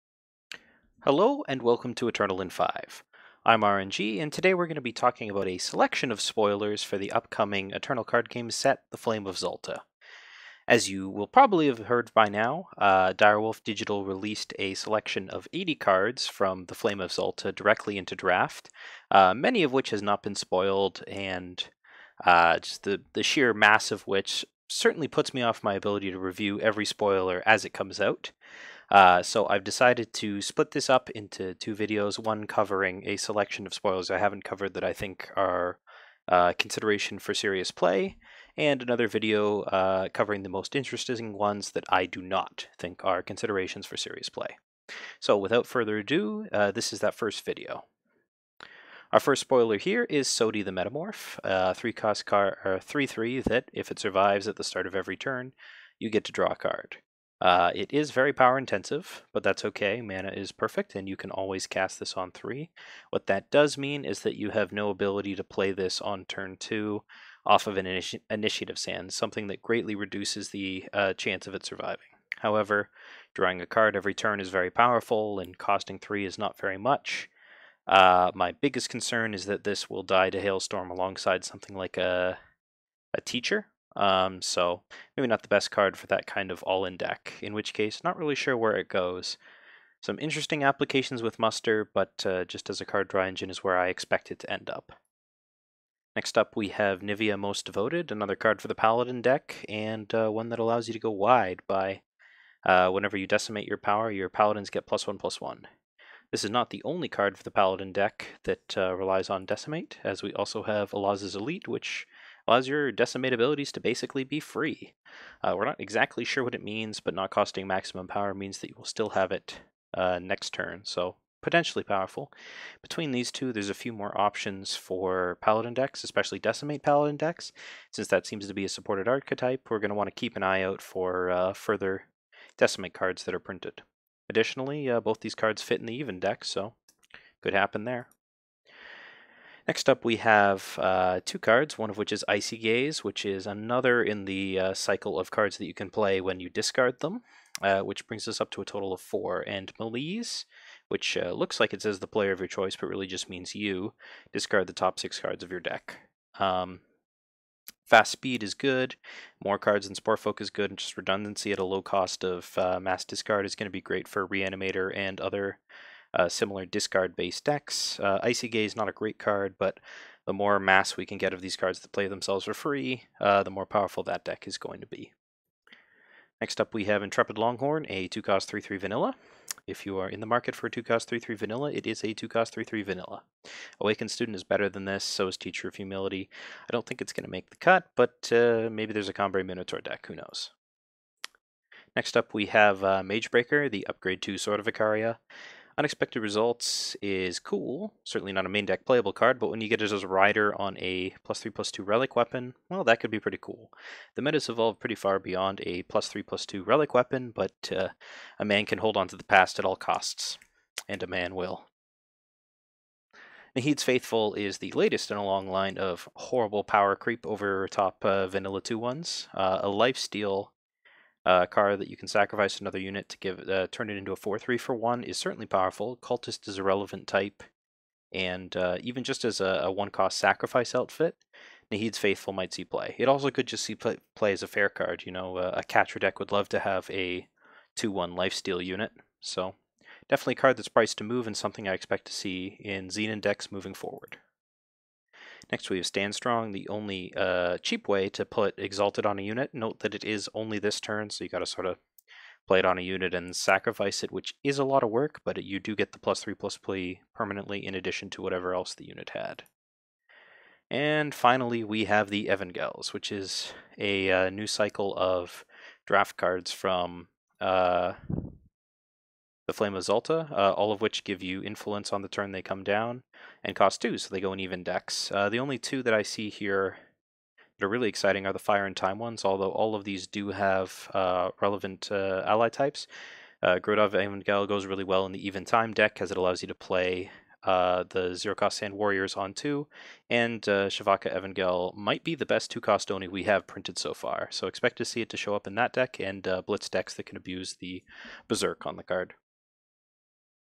Eternal in Five Ep 14 - Potpourri Playables
Too many spoilers to review all in one go! I split them up between this video (the good ones) and Ep 15 (the other ones).